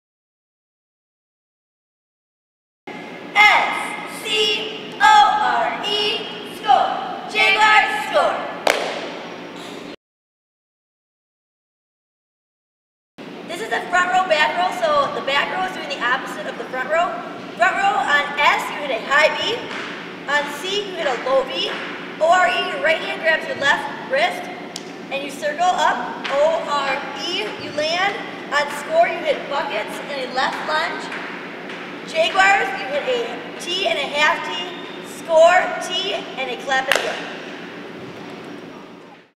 S-C-O-R-E, Score, Jaguars, Score!
Words and movements to the cheer "S-C-O-R-E, Score, Jaguars, Score!''